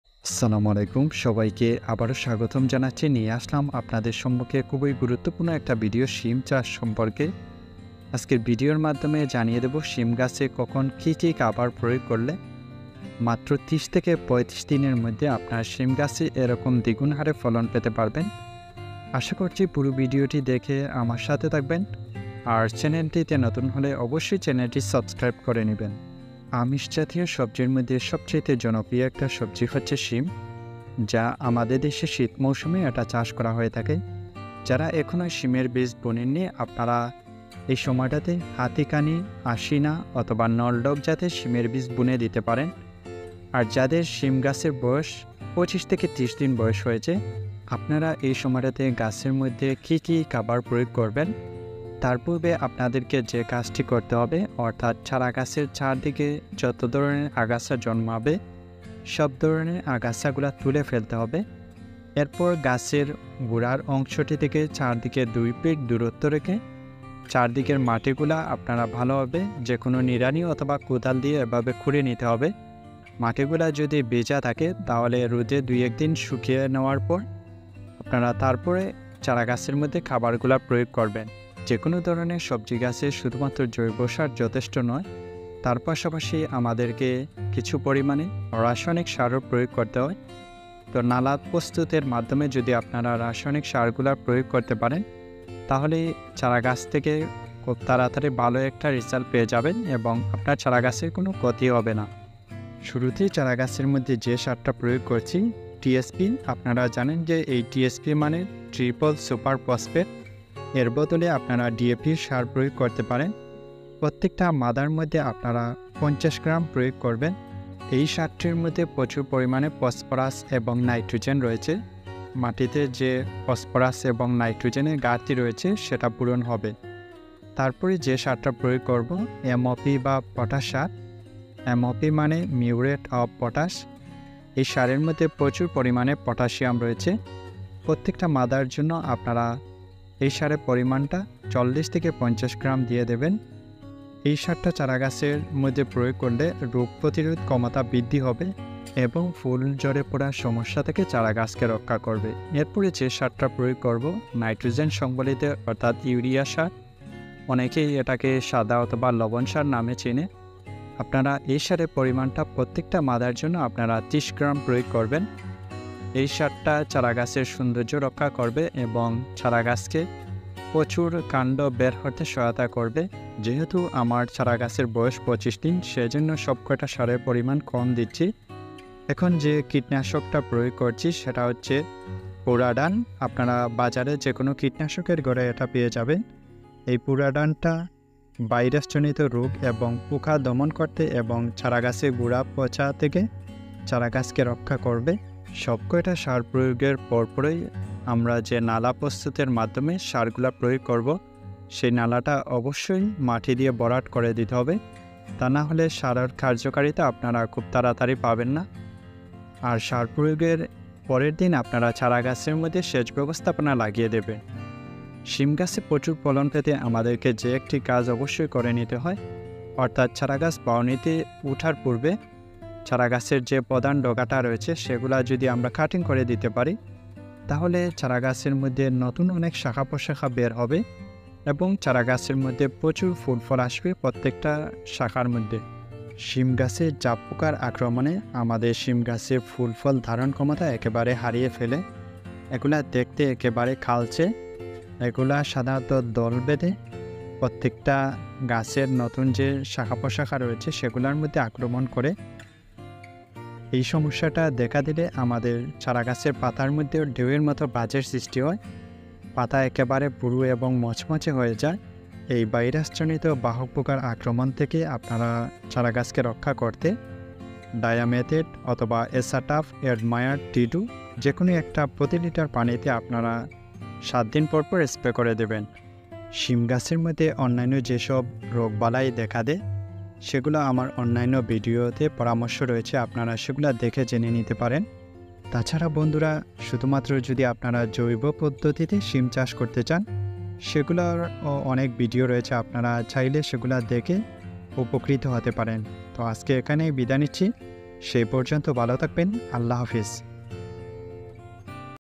শিম গাছে কি কি খাবার দিলে গাছে দ্রুত প্রচুর শিম ধরবে। শিম গাছের পরিচর্যা। Beans plant care
আসসালামু আলাইকুম কৃষি ভিত্তিক চ্যানেলে আপনাকে স্বাগতম 🌹❤️
আজকের ভিডিও তে রয়েছে শিম গাছে কি কি খাবার দিলে গাছে দ্রুত প্রচুর শিম ধরবে। আশা করছি পুরো ভিডিও টি দেখে উপকৃত হবেন।